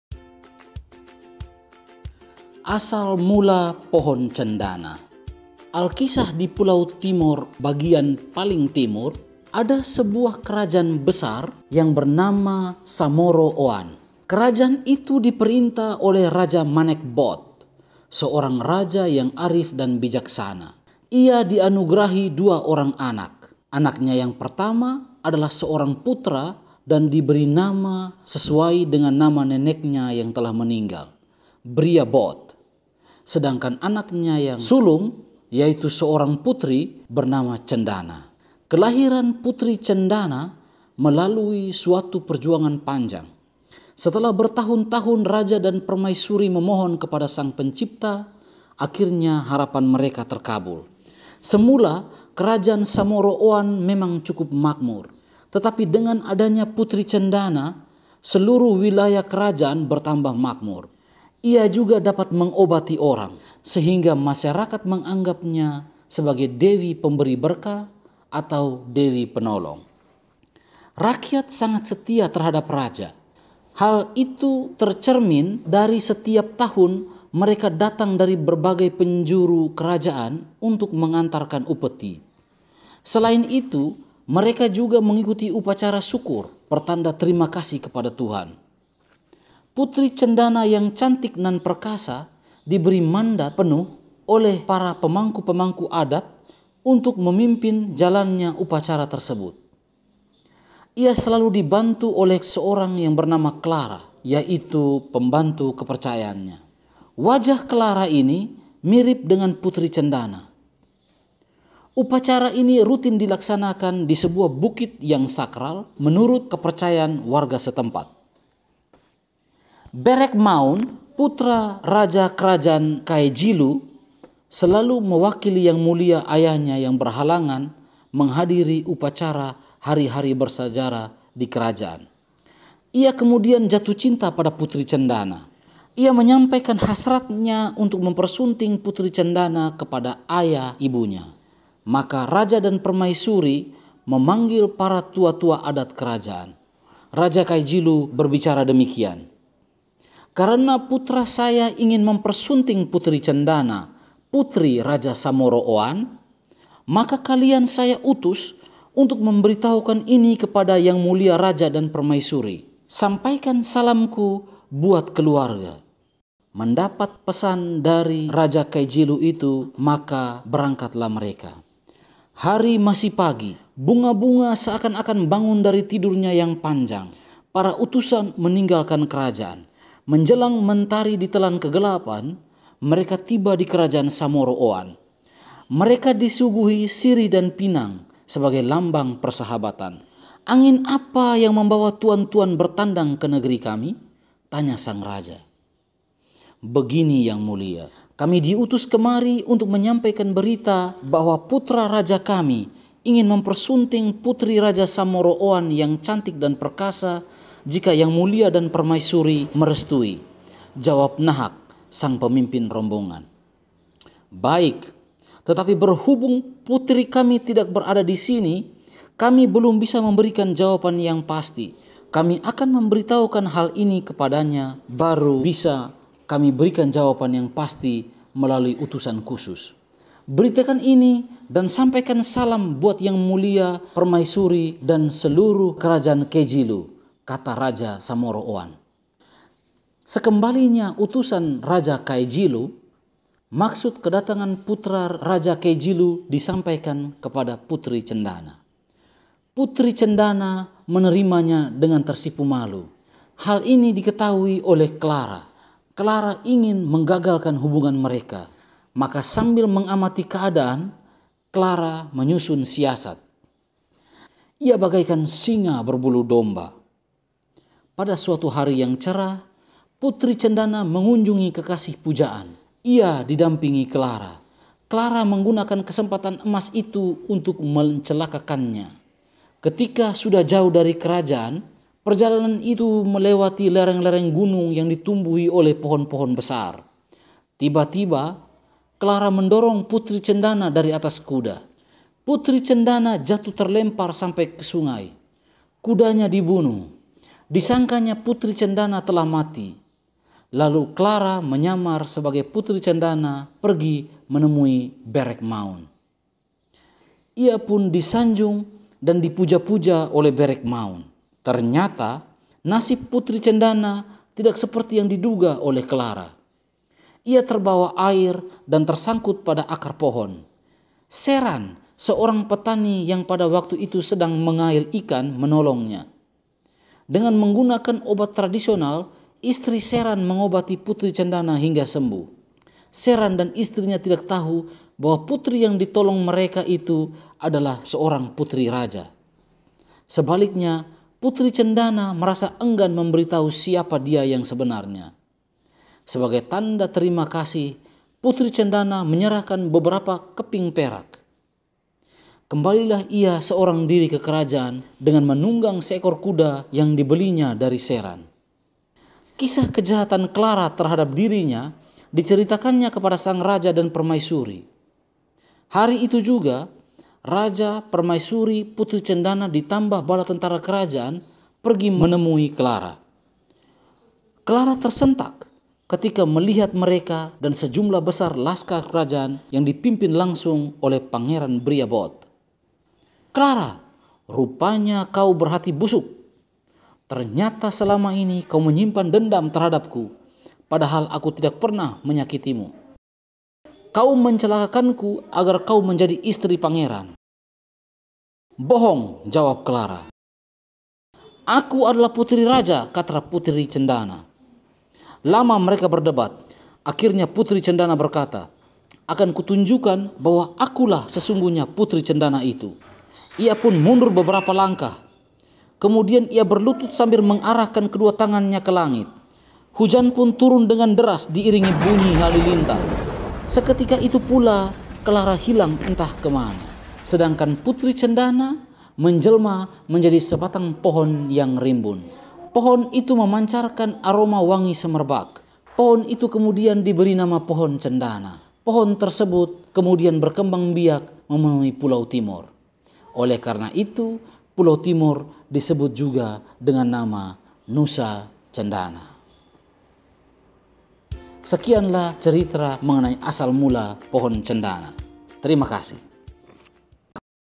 ASAL MULA POHON CENDANA
Video ini berisi cerita dongeng kemunculan pohon cendana berdasarkan versi dari daerah Malaka, Timor.